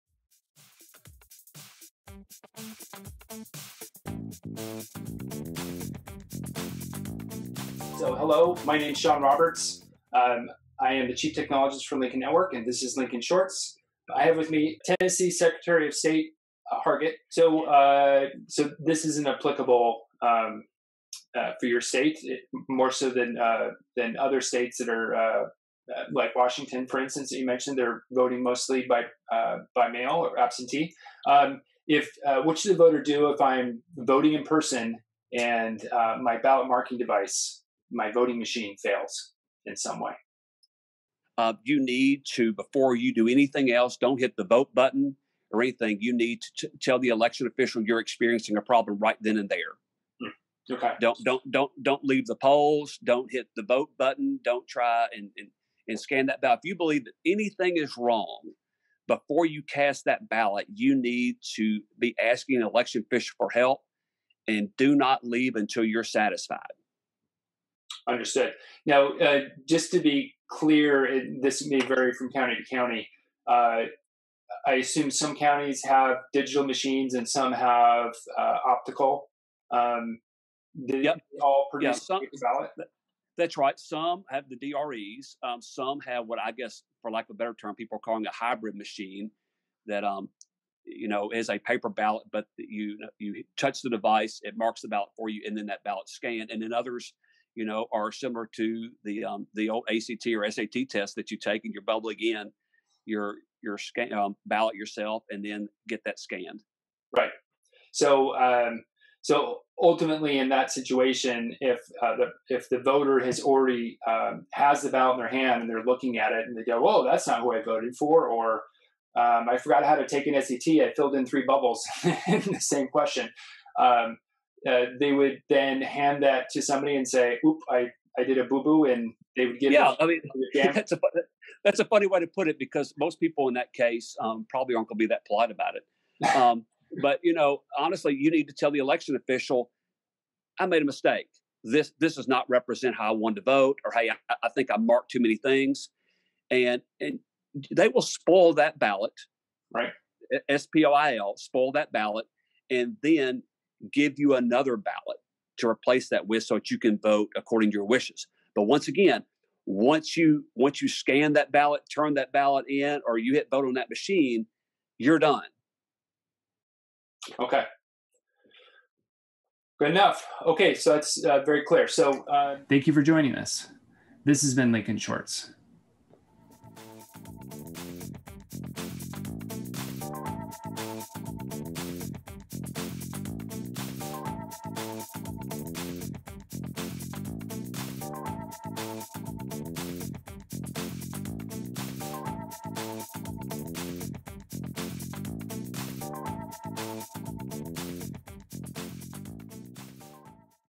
Lincoln Shorts Ep 32: Tre Hargett, What Should the Voter Do if my ballot marking device fails
Tre Hargett, Tennessee Secretary of State talks with Sean on what Should the voter do if I am voting in-person and my ballot marking device fails?  We note some of the details of spoiled ballots.